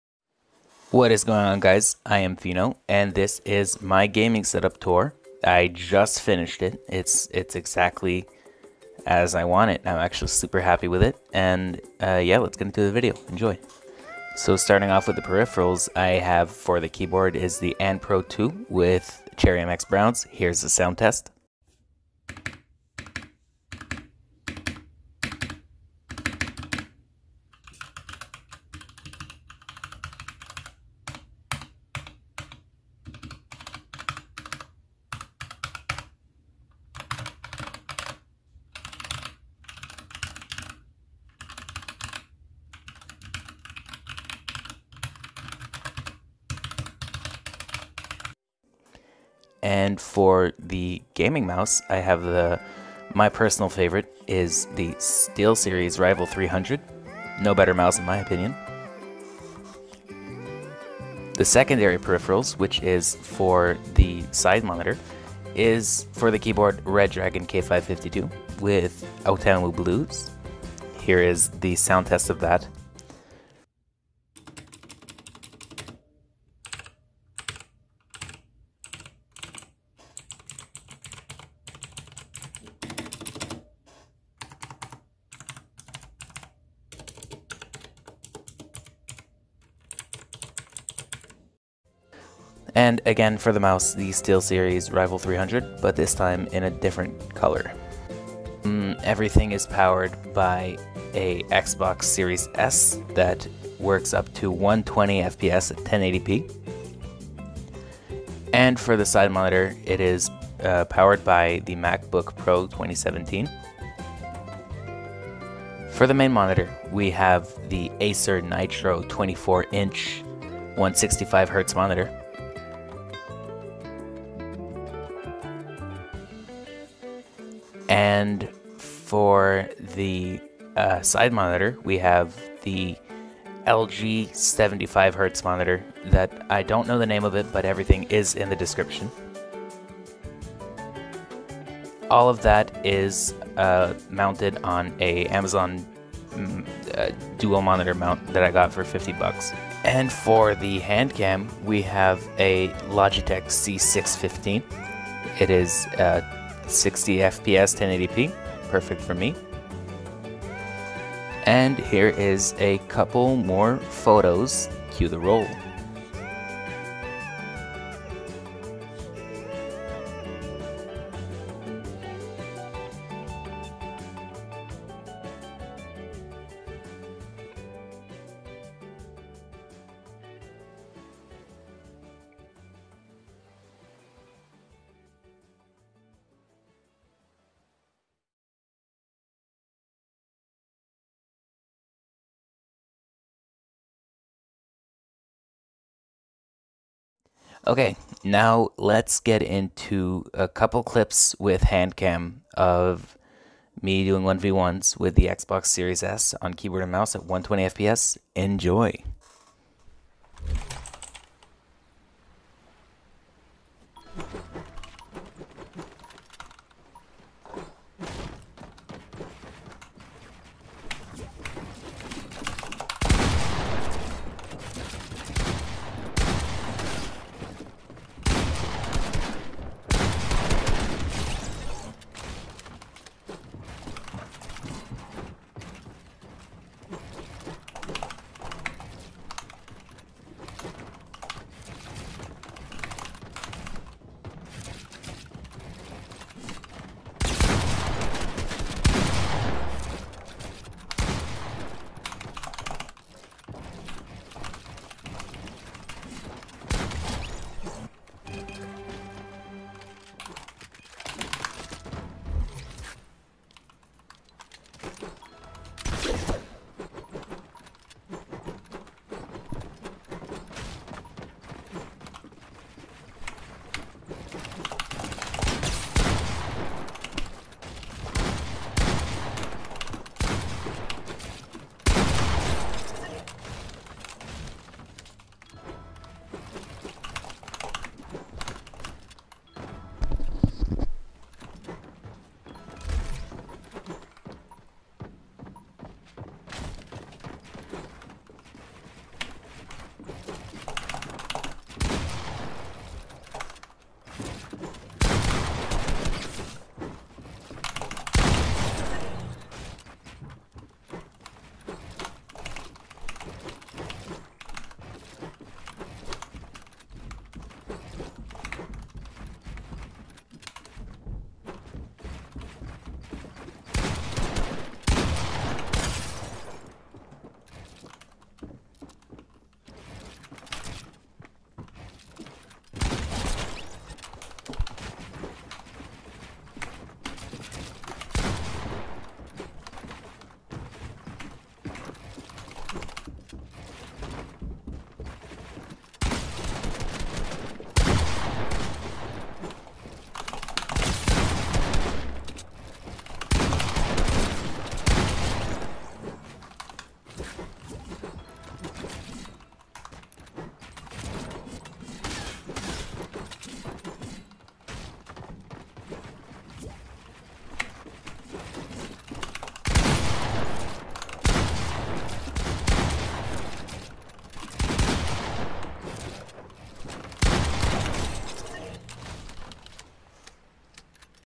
Xbox Series S 120FPS Gaming Setup Tour + Keyboard And Mouse 120FPS Gameplay
ITEMS IN MY GAMING SETUP:

Anne Pro II
Redragon K552
SteelSeries Rival 300
Acer Nitro 24" 165Hz Gaming Monitor
LG 22" 75Hz Gaming Monitor
HIHUHEN Gaming Mouse Pad (eagle not available)
Dual Monitor Mount
MacBook PRO 3-Port USB Adapter